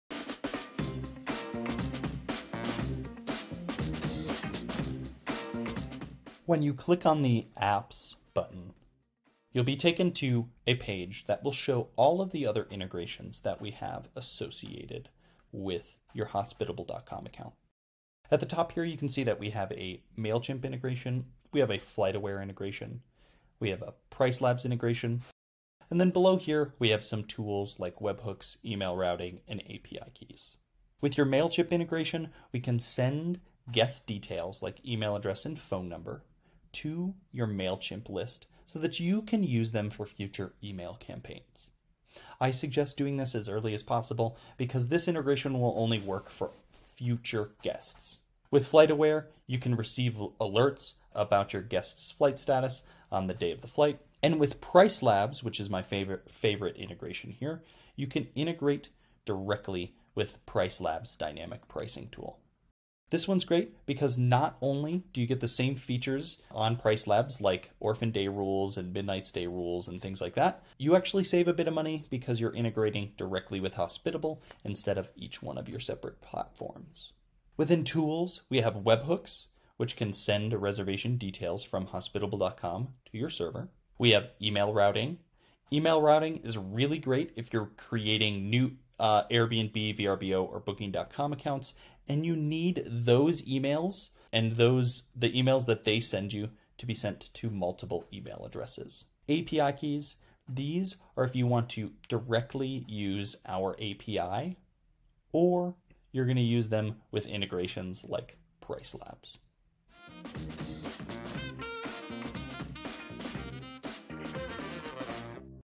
Hospitable training: Getting Started with Apps
Check out our training video to teach you how to use Hospitable's integrations with all your favourite apps.